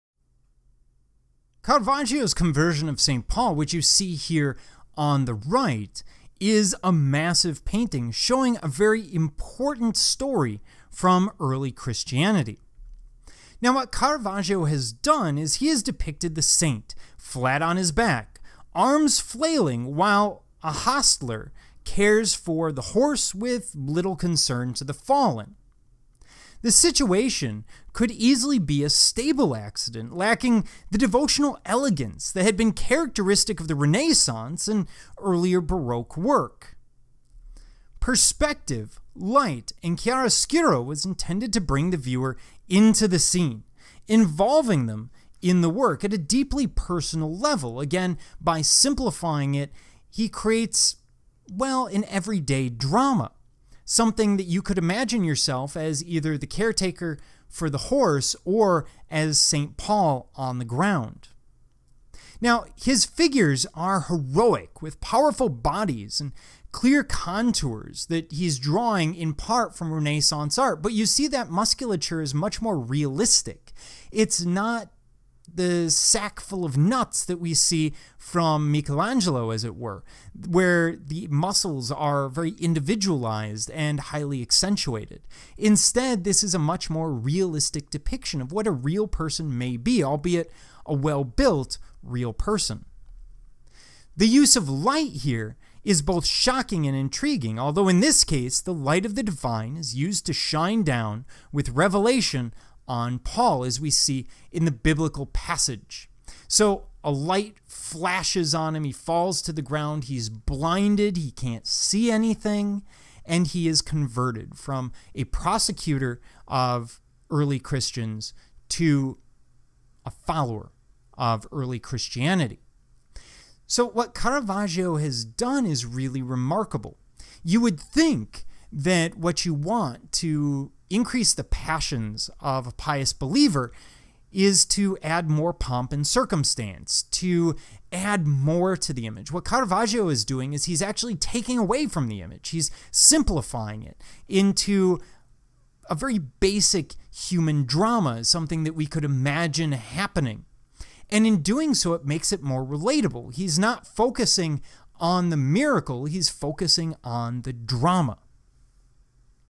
Caravaggio Conversion of Saint Paul
A brief lecture on Caravaggio's Conversion of Saint Paul